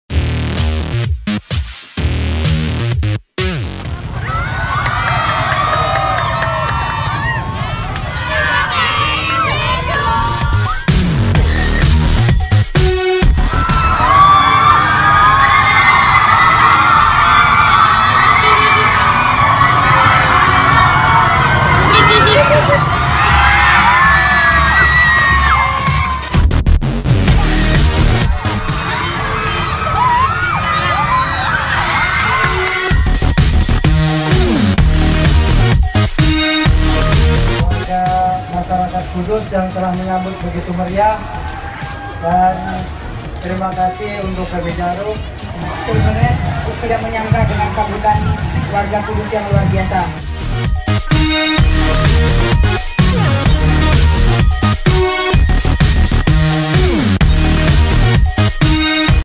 ANTARANEWS - KEMERIAHAN KIRAB TONTOWI/LILIYANA DI KUDUS
Peraih medali emas Olimpiade 2016 Tontowi Ahmad/Liliyana Natsir disambut sangat meriah oleh masyarakat Kudus, Kamis, yang begitu semangat menyambut dua pahlawan olahraga Indonesia itu, mengalahkan sambutan warga ibu kota Jakarta saat pasangan ini diarak beberapa waktu lalu.

Video: ANTARA News/ Monalisa